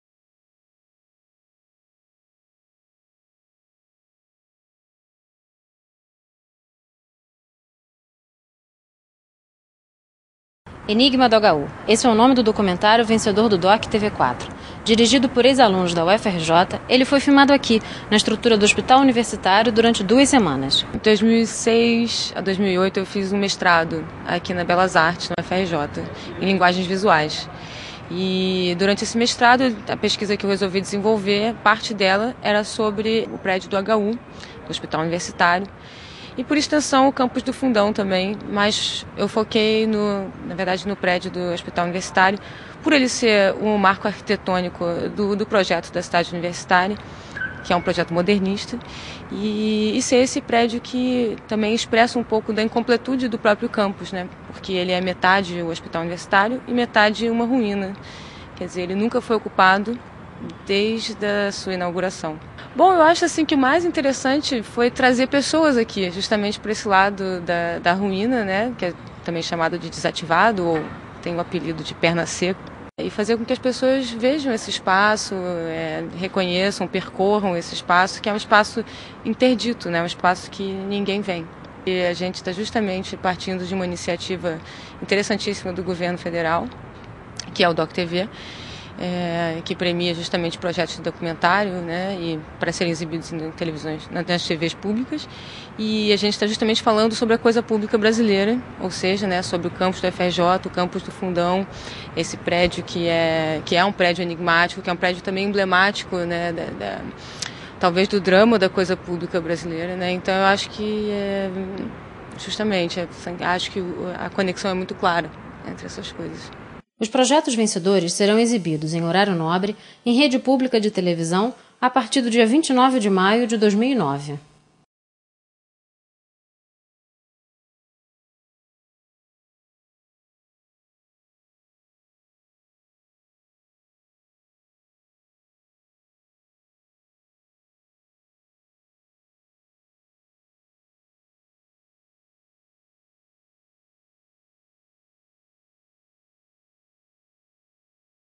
09/12/2008 - Enigma do HU
Enigma do HU

Esse é o nome do documentário vencedor do DOCTV4. Dirigido por ex-alunos da UFRJ, o documentário foi filmado, durante duas semanas, na parte desativada do hospital, conhecida como perna seca.
Lançado em agosto de 2003, o programa DOCTV é uma iniciativa pioneira realizada por meio de convênio entre a Secretaria do Audiovisual do Ministério da Cultura, TV Cultura e a Associação Brasileira das Emissoras Públicas, Educativas e Culturais (ABEPEC). Neste ano 35 produções da área audiovisual foram premiadas.
A partir de maio do ano que vem os documentário serão exibidos, em horário nobre, na rede pública de televisão.